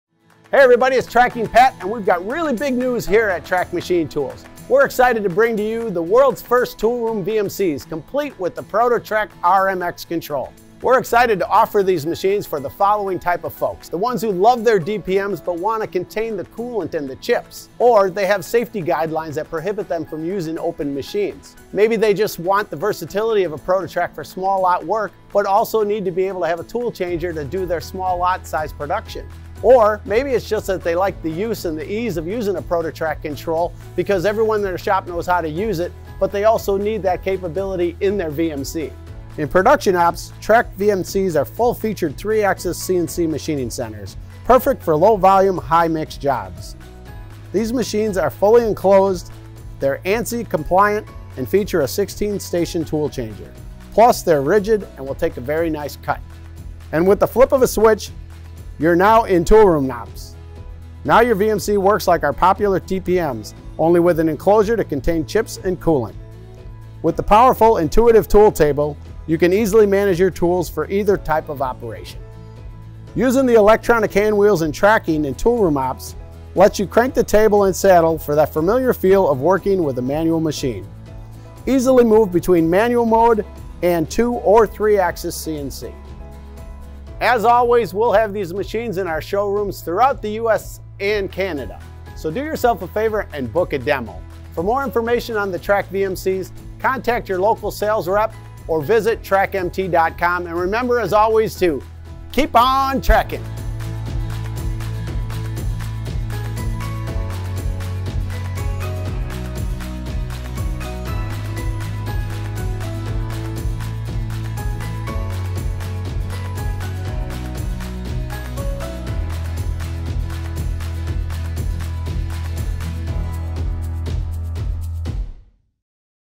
Introducing The World's First Toolroom Machining Center
In Production Ops, it's a full featured 3-axis machining center when you need to run low volume production. With the flip of a switch, you enter into Toolroom Ops™. Now your TMC works like our popular TRAK DPM, only with an enclosure that keeps chips and coolant contained.